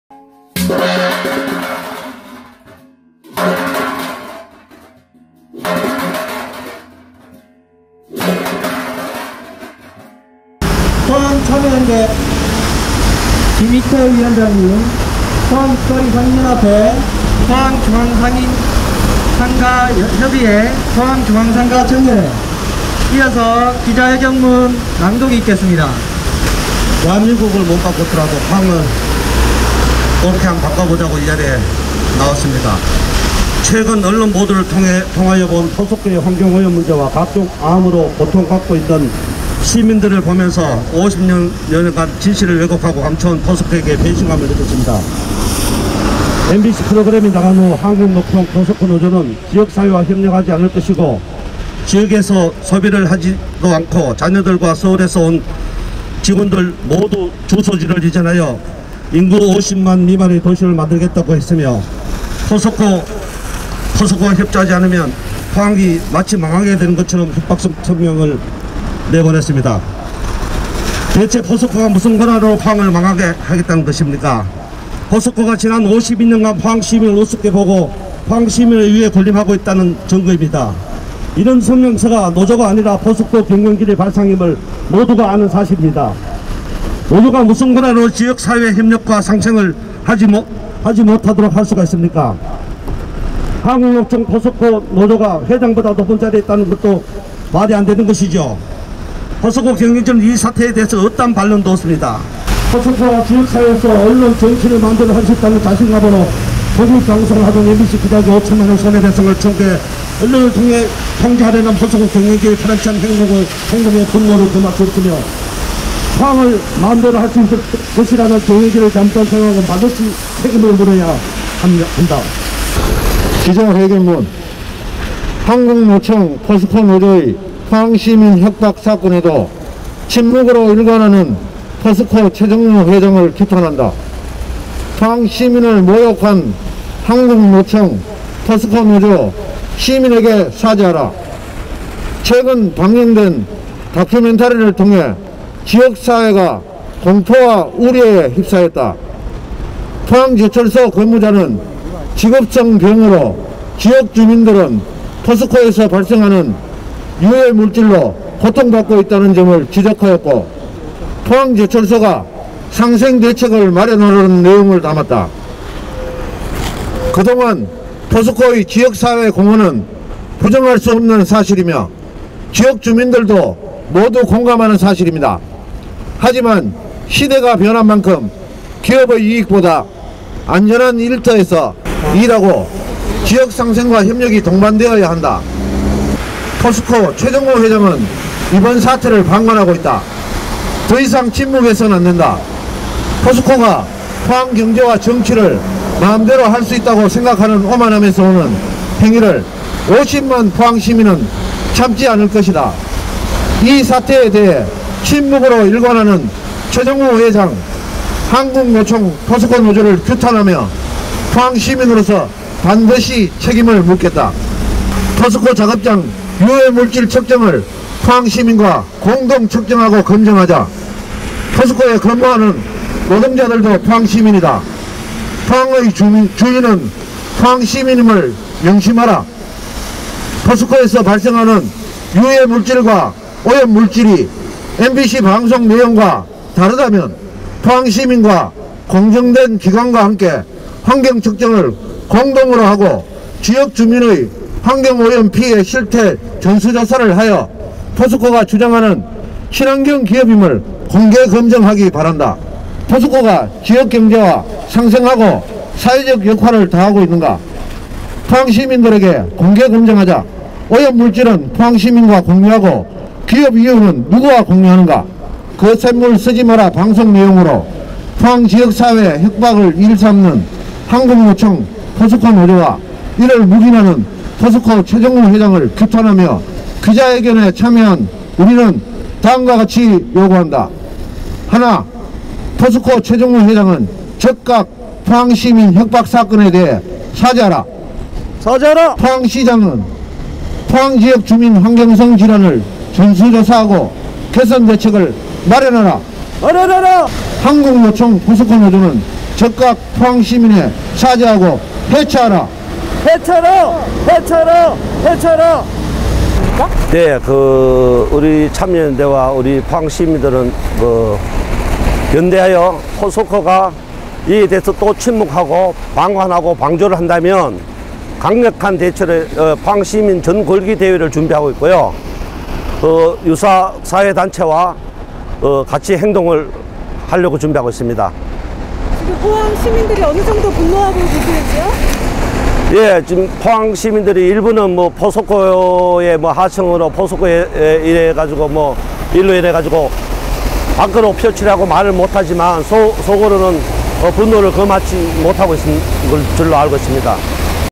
한국노총 포스코 노조 포항시민 협박 사건 사죄 하라!
포스코 최정우 회장 은 당당히 나와 밝혀라!
포스코는 국민의 기업이다. 포항시민 협박하는 포스코는 오만함에서 오는 행위 50만 포항시민은 참지 않을 것이다.
환경 유해물질 공동으로 조사하고 검증하자  지역주민 피해 장기적 대책 마련하라!
포항시는 포항시민 건강 조사 실시하라!